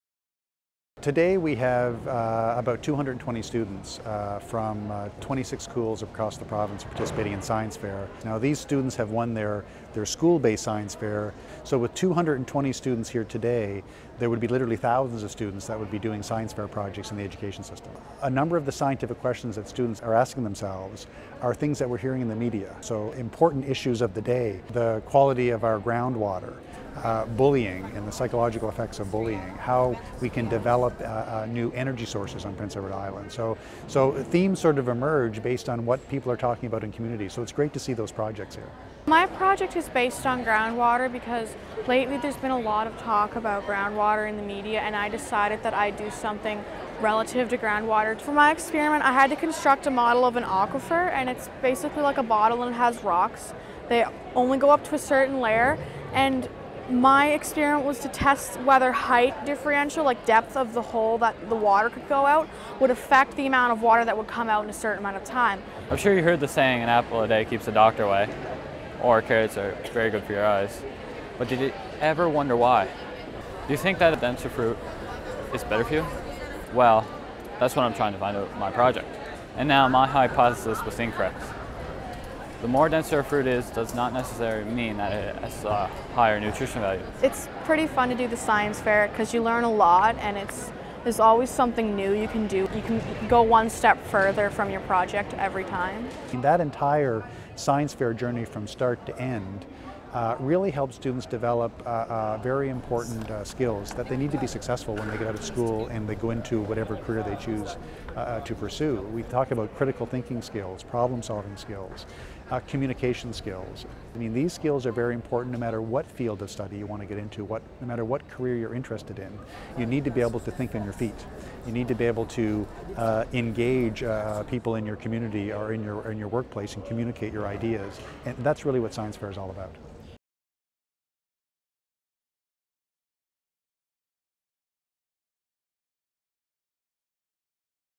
2014 Provincial Science Fair
The 2014 Provincial Science Fair took place at the University of Prince Edward Island on April 3, 2014. The theme of this year's fair was "Science Matters." Students from grades 4--12 showcased their science interests and abilities, and connected with other young scientists from around the province.